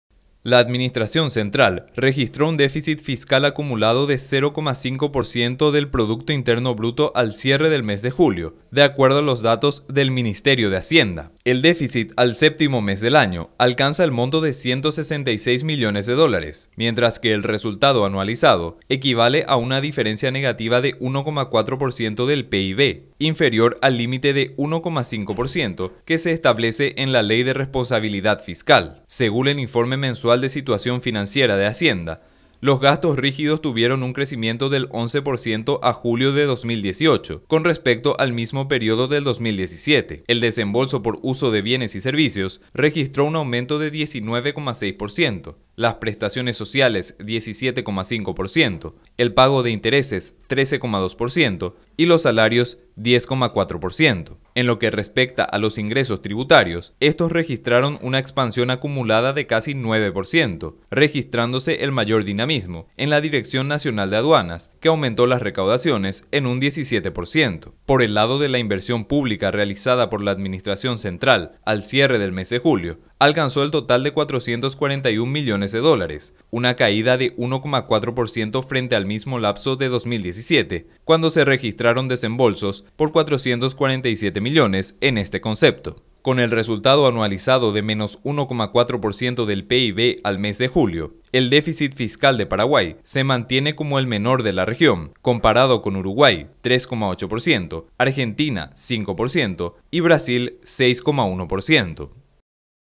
Fisco registra déficit anualizado de 1,4% a julio
La Administración Central registró un déficit fiscal acumulado de 0,5% del producto interno bruto al cierre del mes de julio. El déficit al séptimo mes del año alcanza el monto de USD 166 millones, mientras que el resultado anualizado equivale a una diferencia negativa de 1,4% del PIB